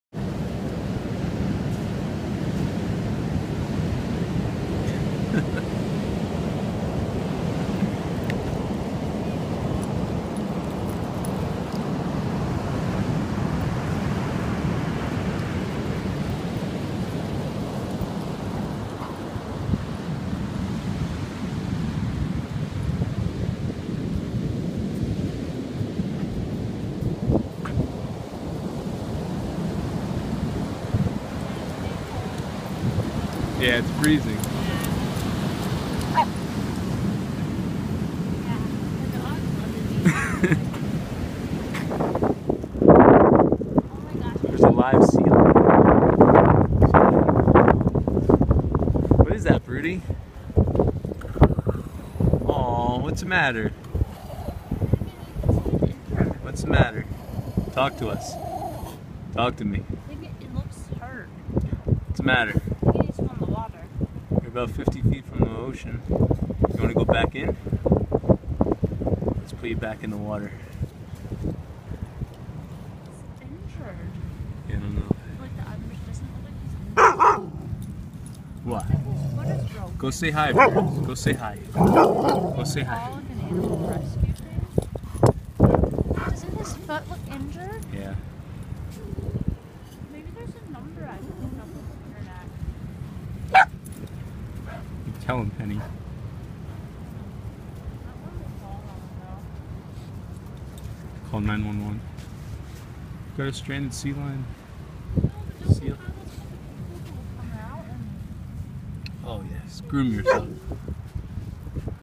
AIREDALE TERRIER Meets a BEACHED SEAL!
Yes, I called the local animal rescue organization and they helped out this hurt seal :)